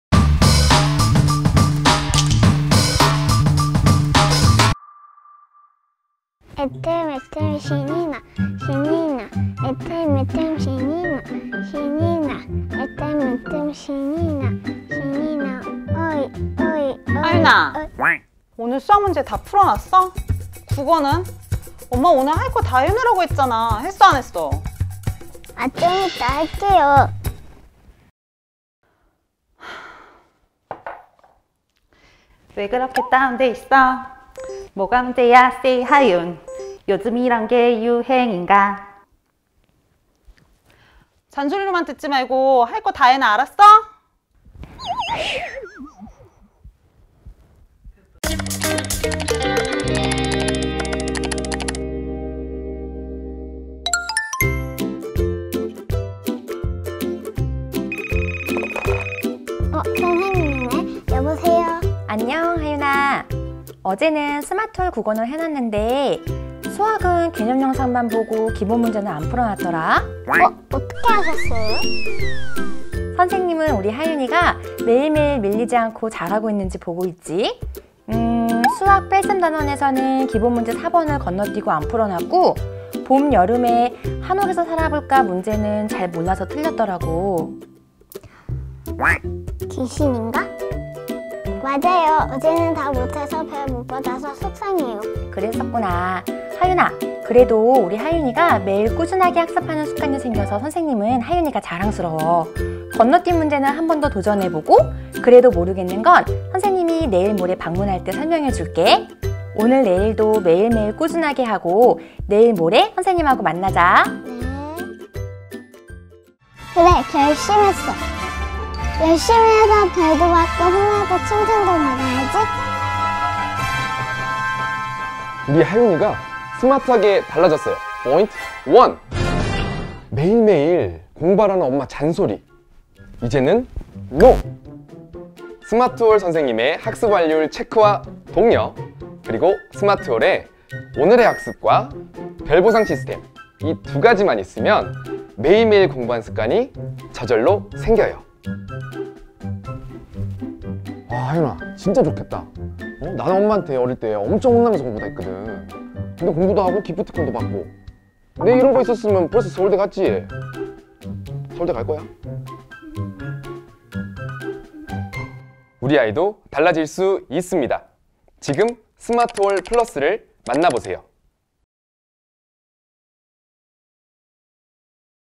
[우리아이 교육성형 ①] 요즘 숙제를 안하는게 유행인가?!
우리 하윤이가 스마트하게 달라졌어요!
Episode 1. 매일학습편
* 교육성형 Before : 매일매일 공부하라는 엄마의 잔소리
* 교육성형 After : 스마트올플러스 선생님의 학습완료율 체크와 독려!
그리고 스마트올의 '오늘의학습'과 '별 보상 시스템'만 있으면 매일 공부하는 습관이 생겨요!

우리 아이도 달라질 수 있습니다!
지금 스마트올플러스를 만나보세요!